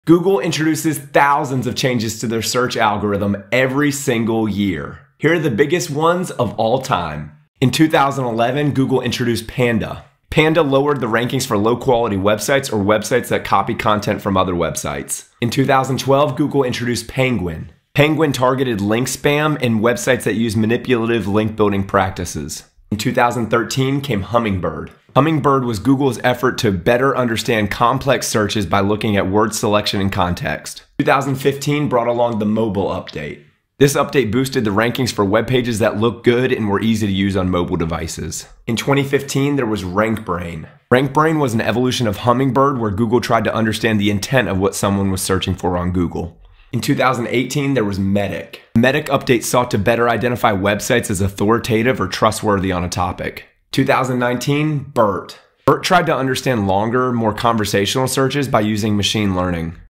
BIGGEST Google Algorithm Updates EVER
Google changes its search algorithm thousands of times per year. Some updates are game-changing and change the search landscape forever. This video will run through the most significant algorithm changes that include updates like Penguin and Panda.